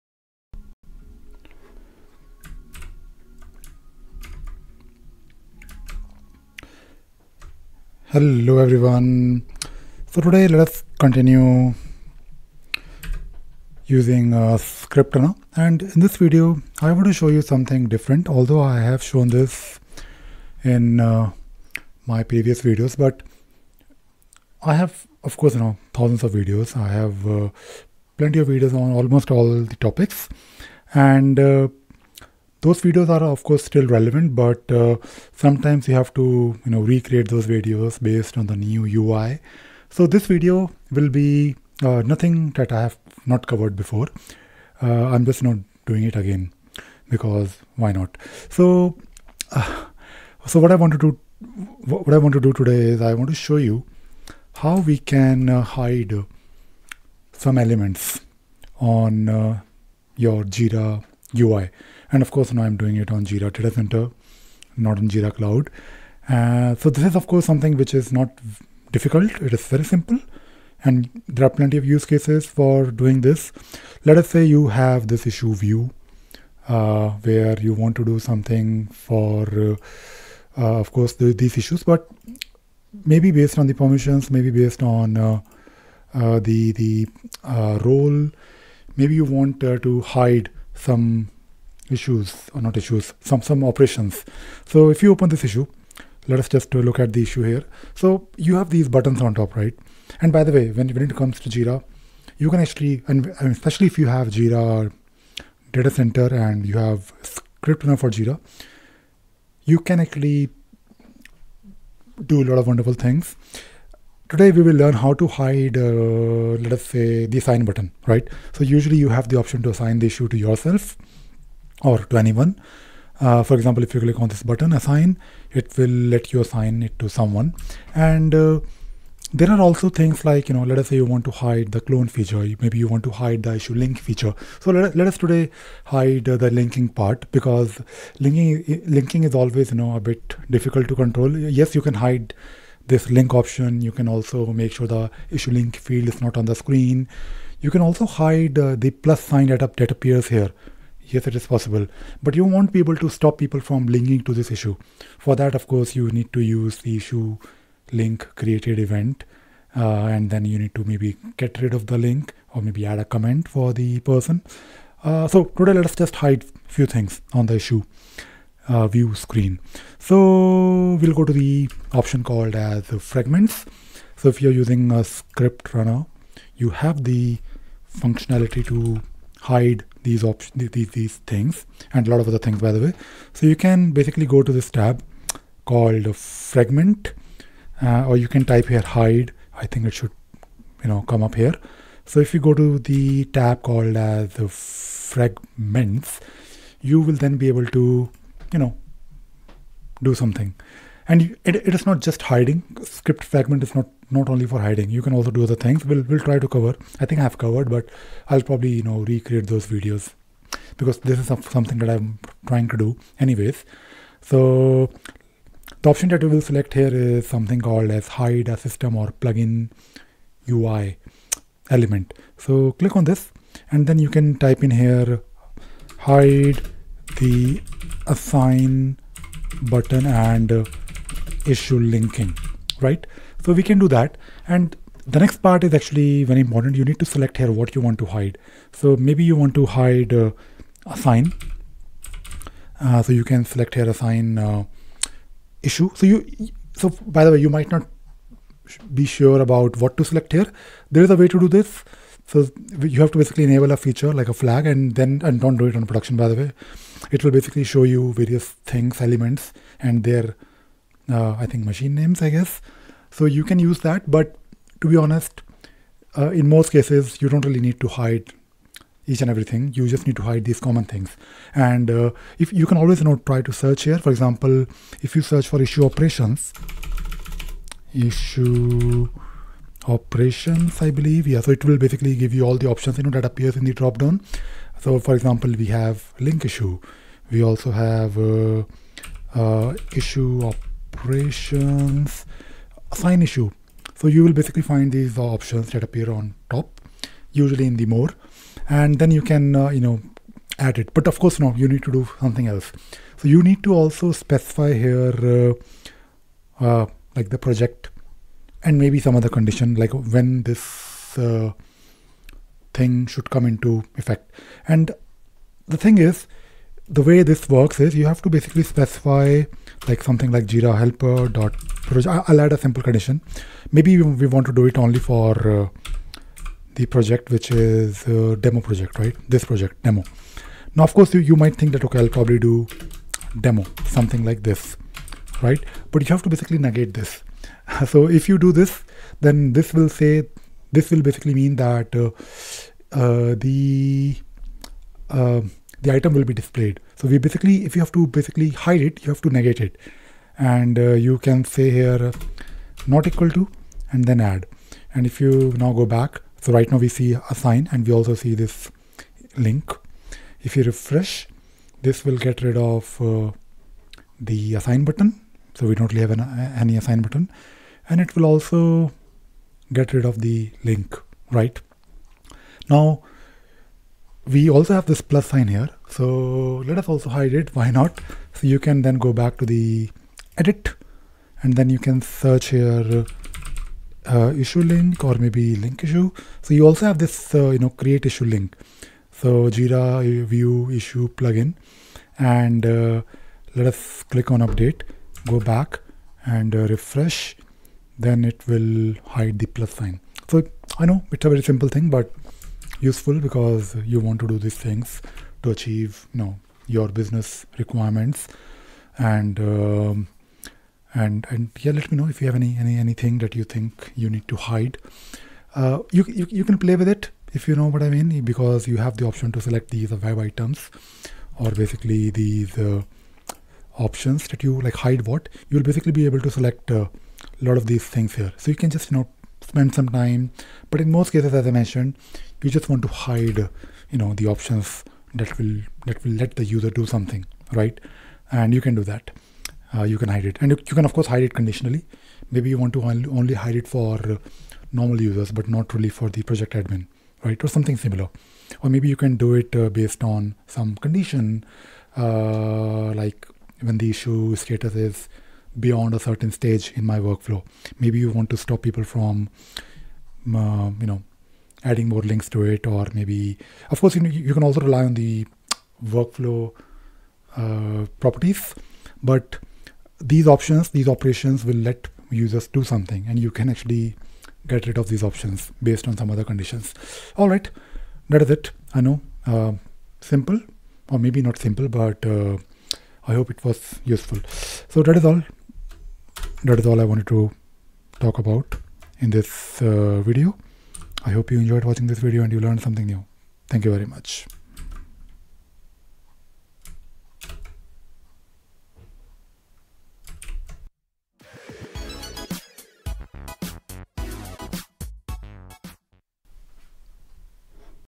Scriptrunner for Jira - Fragments hide Assign button and Issue Linking
You can easily hide assign buttons, issue linking operations and lot of other web items from your Jira UI based on a condition.